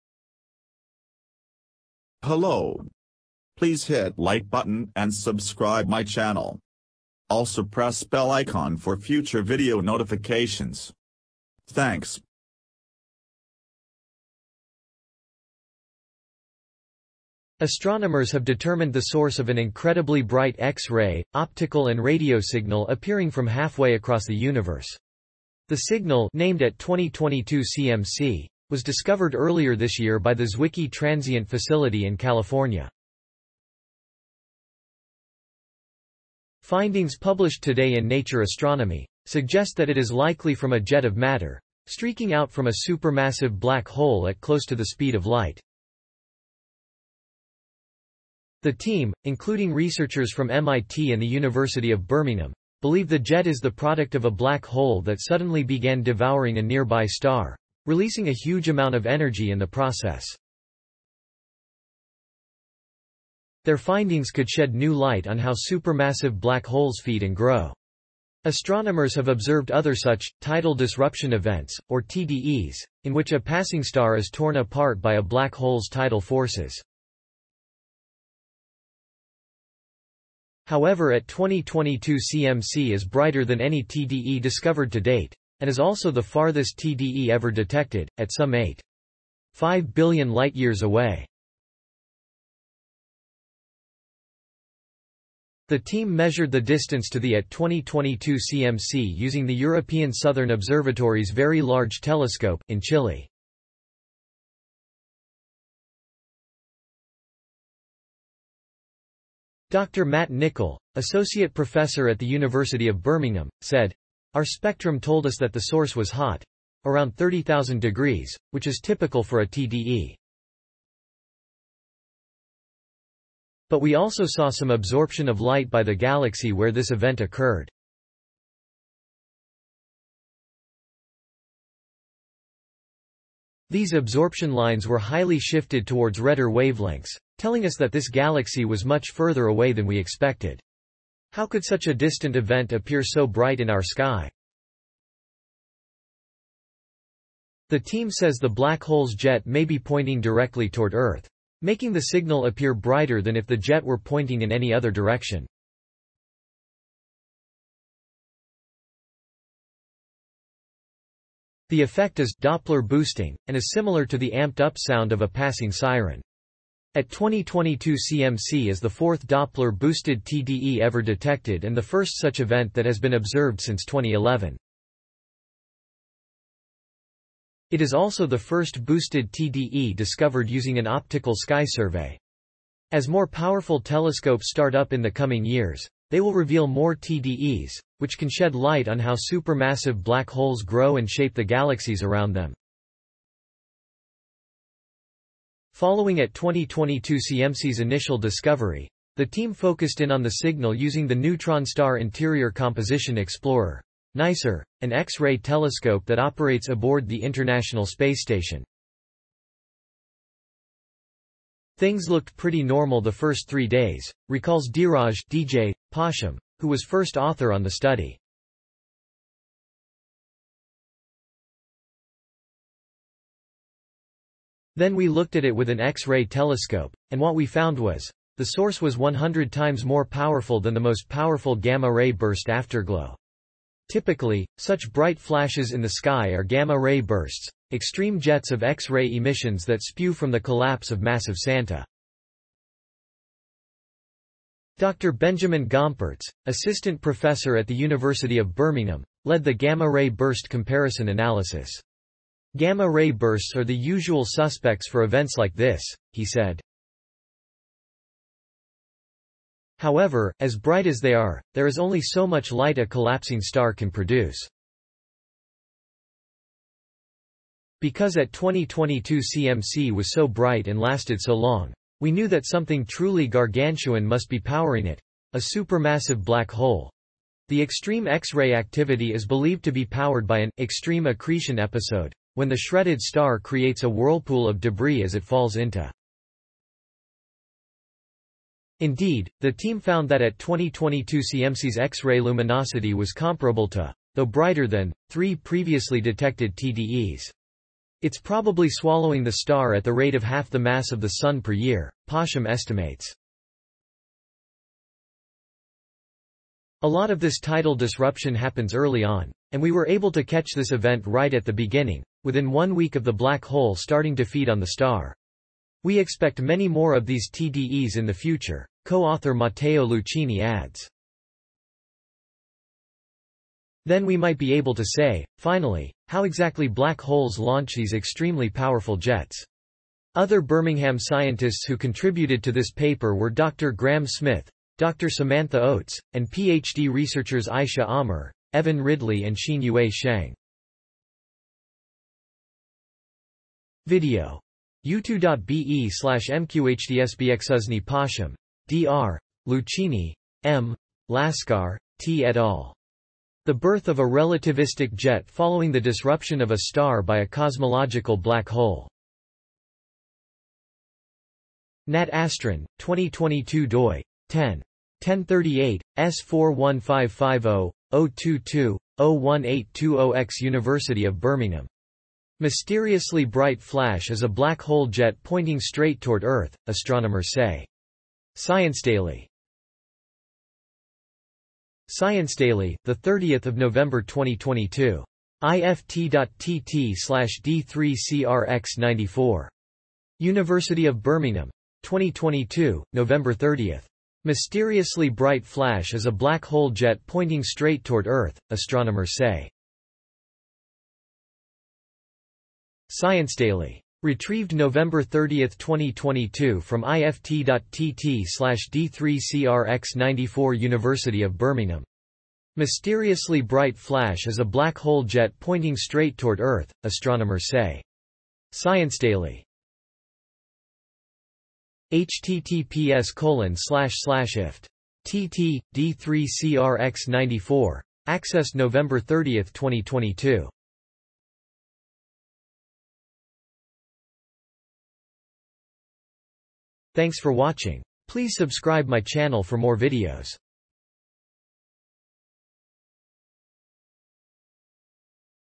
Mysteriously bright flash is a black hole jet pointing straight toward Earth, astronomers say
Astronomers have determined the source of an incredibly bright X-ray, optical and radio signal appearing from halfway across the Universe. The signal, named AT 2022cmc, was discovered earlier this year by the Zwicky Transient Facility in California. Findings published today in Nature Astronomy, suggest that it is likely from a jet of matter, streaking out from a supermassive black hole at close to the speed of light.
The team, including researchers from MIT and the University of Birmingham, believe the jet is the product of a black hole that suddenly began devouring a nearby star, releasing a huge amount of energy in the process. Their findings could shed new light on how supermassive black holes feed and grow.
Astronomers have observed other such “tidal disruption events,” or TDEs, in which a passing star is torn apart by a black hole’s tidal forces. However AT 2022cmc is brighter than any TDE discovered to date, and is also the farthest TDE ever detected, at some 8.5 billion light years away.
The team measured the distance to the AT 2022cmc using the European Southern Observatory’s Very Large Telescope, in Chile.
Dr Matt Nicholl, associate professor at the University of Birmingham, said: “Our spectrum told us that the source was hot: around 30,000 degrees, which is typical for a TDE. But we also saw some absorption of light by the galaxy where this event occurred. These absorption lines were highly shifted towards redder wavelengths, telling us that this galaxy was much further away than we expected!”
How could such a distant event appear so bright in our sky? The team says the black hole’s jet may be pointing directly toward Earth, making the signal appear brighter than if the jet were pointing in any other direction. The effect is “Doppler boosting,” and is similar to the amped-up sound of a passing siren.
AT 2022cmc is the fourth Doppler-boosted TDE ever detected and the first such event that has been observed since 2011. It is also the first boosted TDE discovered using an optical sky survey. As more powerful telescopes start up in the coming years, they will reveal more TDEs, which can shed light on how supermassive black holes grow and shape the galaxies around them.
Following AT 2022cmc’s initial discovery, the team focused in on the signal using the Neutron star Interior Composition ExploreR (NICER), an X-ray telescope that operates aboard the International Space Station.
“Things looked pretty normal the first three days,” recalls Dheeraj “DJ” Pasham, who was first author on the study. “Then we looked at it with an X-ray telescope, and what we found was, the source was 100 times more powerful than the most powerful gamma-ray burst afterglow.”
Typically, such bright flashes in the sky are gamma-ray bursts — extreme jets of X-ray emissions that spew from the collapse of massive stars.
Dr Benjamin Gompertz, assistant professor at the University of Birmingham, led the gamma-ray burst comparison analysis. “Gamma-ray bursts are the usual suspects for events like this.” he said. “However, as bright as they are, there is only so much light a collapsing star can produce. Because AT 2022cmc was so bright and lasted so long, we knew that something truly gargantuan must be powering it — a supermassive black hole.”
The extreme X-ray activity is believed to be powered by an “extreme accretion episode” when the shredded star creates a whirlpool of debris as it falls into the black hole. Indeed, the team found that AT 2022cmc’s X-ray luminosity was comparable to, though brighter than, three previously detected TDEs.
“It’s probably swallowing the star at the rate of half the mass of the sun per year,” Pasham estimates. “A lot of this tidal disruption happens early on, and we were able to catch this event right at the beginning, within one week of the black hole starting to feed on the star.”
“We expect many more of these TDEs in the future,” co-author...